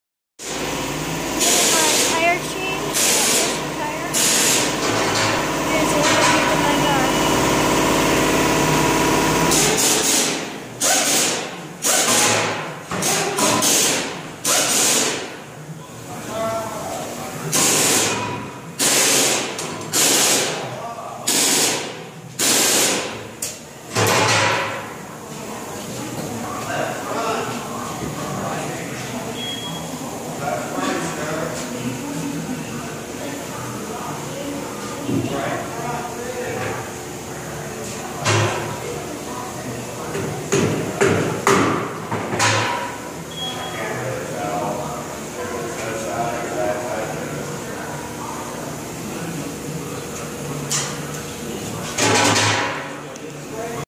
Daily Driver in Ohio Winters
My Ford Focus 2006. Getting 4 new tires and an oil change
What a daily driver looks like in Ohio (no garage)